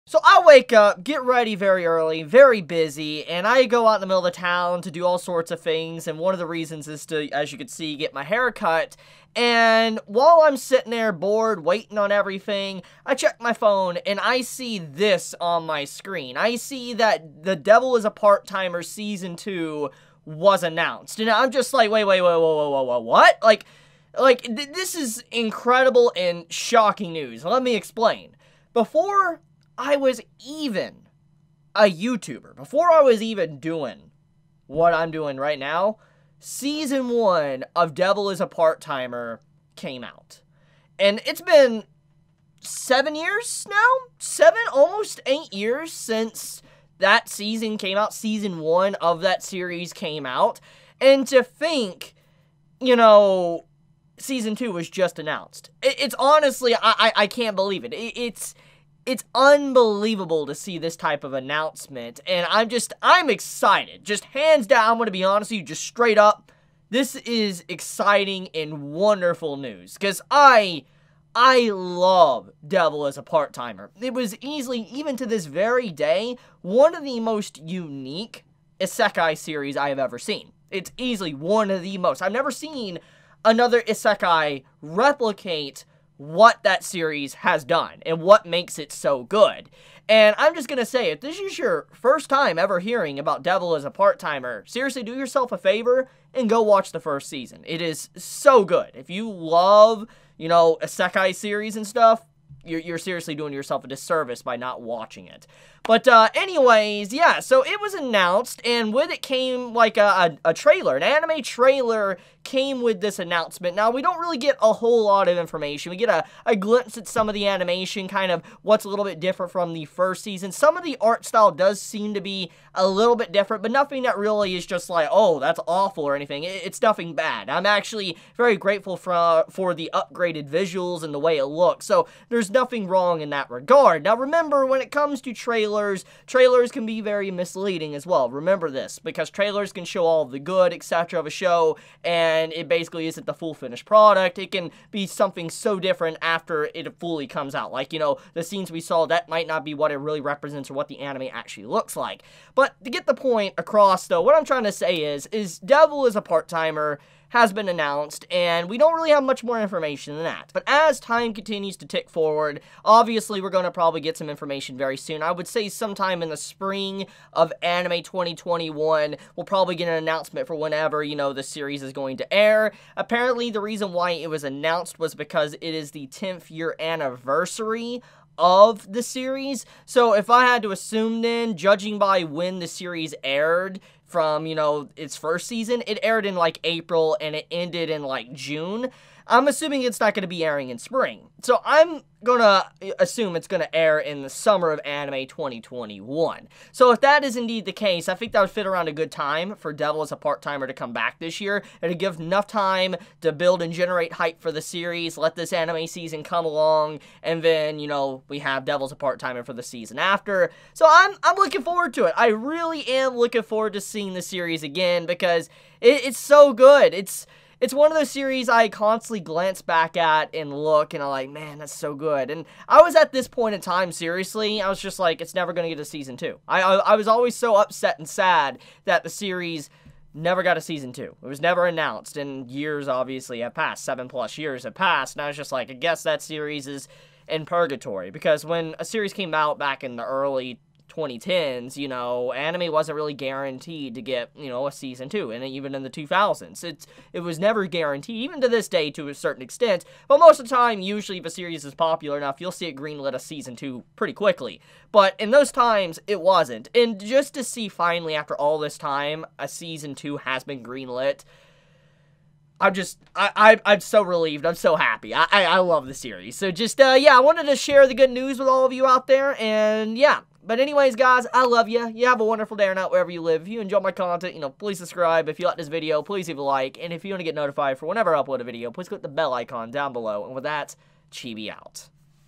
The Devil is a Part Timer Gets a Season 2 Announced After 8 Years
Long before I was really a YouTuber... The Devil is a Part Timer came out. It aired back in Spring of Anime 2013 and its been almost 8 years since then. For its 10th Anniversary of the Light Novel... Season 2 of the series has been announced. We got a trailer and found out the original cast is returning.

For any fellow Chibit that wants to send something:
205 Doug Luna Road, 38587, Walling, TN